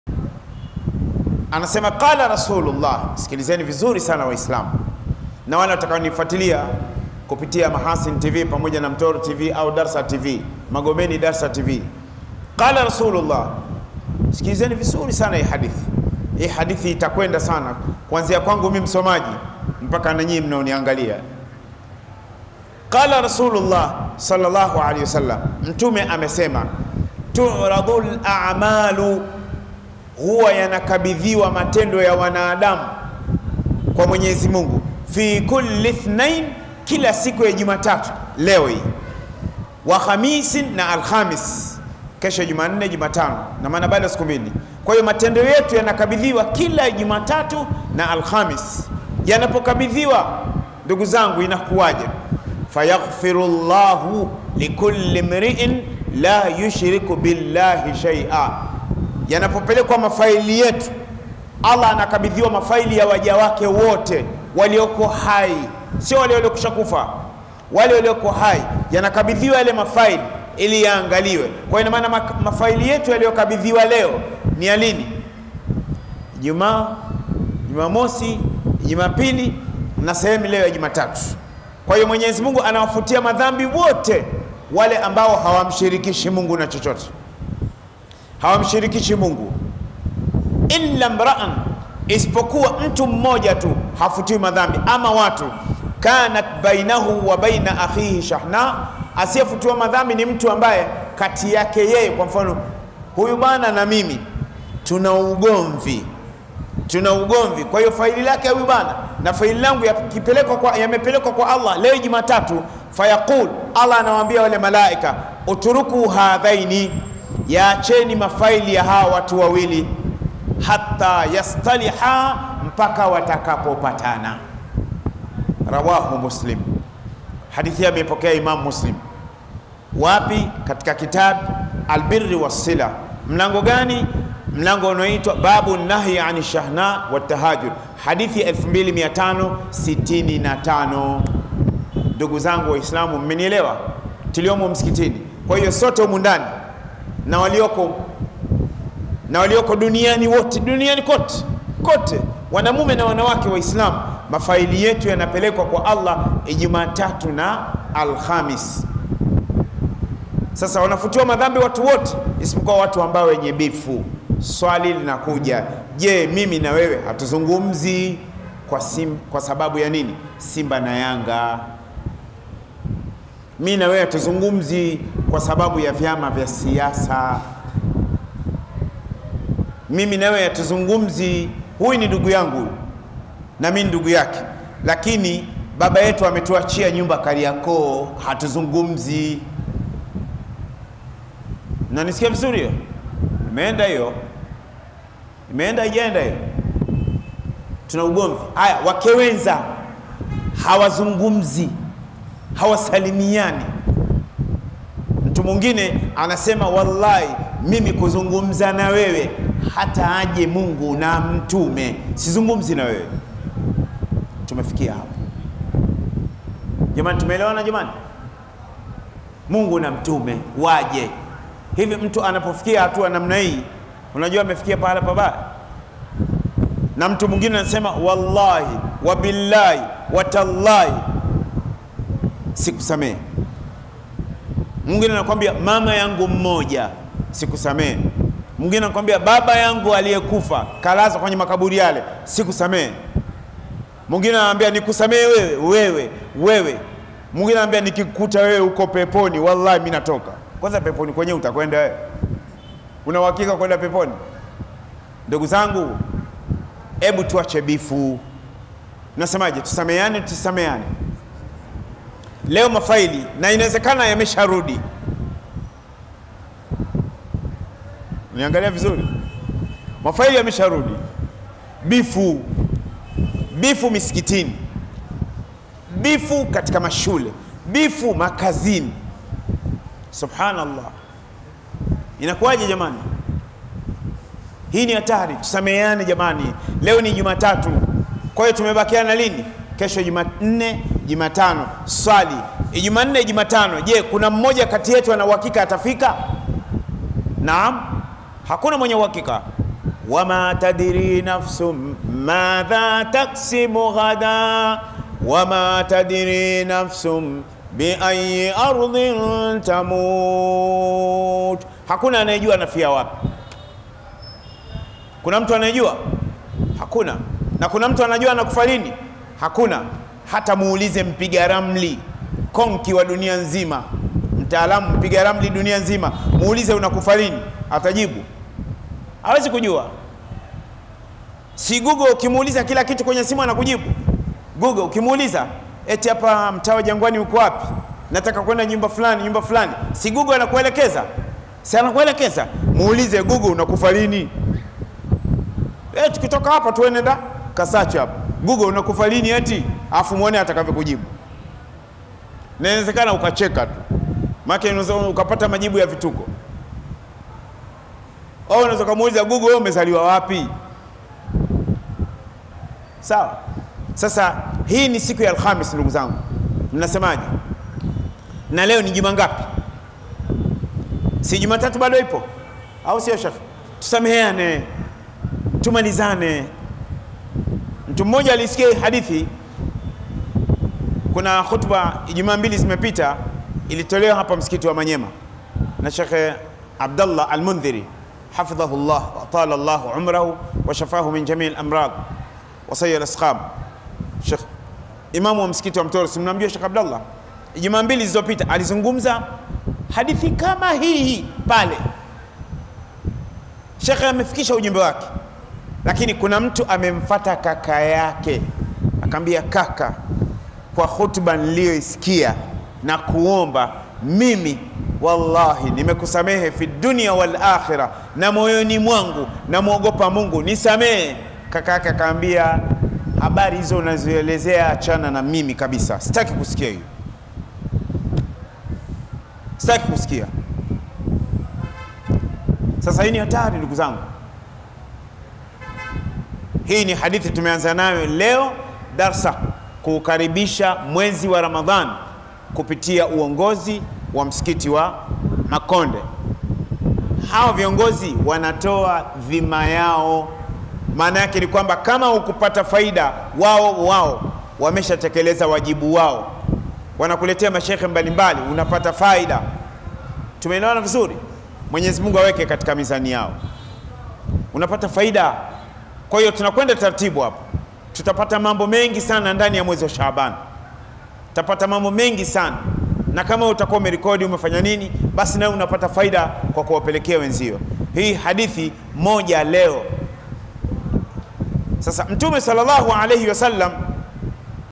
Allah Anafuta Madhambi Siku 2 Hizi / Tusameane Jamani Tuache Bifu / Sheikh Hashimu Rusaganya
Mashaa Allah Sheikh Hashimu Rusaganya  akielezea kwa uzuri na ubora wa hali ya juu Ya Allah Anafuta Madhambi Siku 2 Hizi / Tusameane Jamani Tuache Bifu / Sheikh Hashimu Rusaganya

And
Thanks